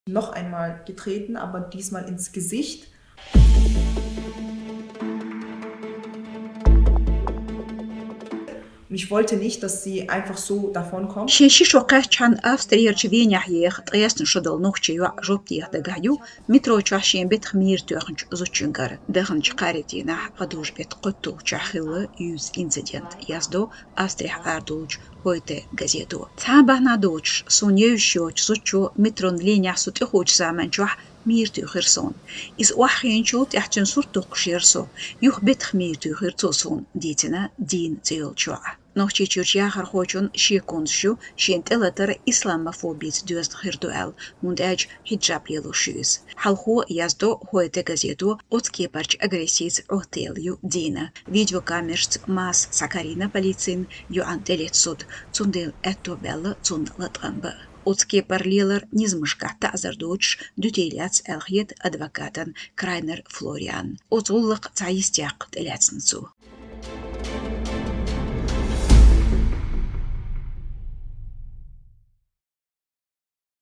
Венехь 19 шо долчу йоIана тIелетта
Австрин Венехь еха 19 шо долу нохчийн йоI жоп деха дагахь ю, метро чохь шена тIелеттачу зудчуьнгара.  Цуьнан шеконаш ю, шена тIелатар исламофобица доьзна хир ду аьлла. ХIунда?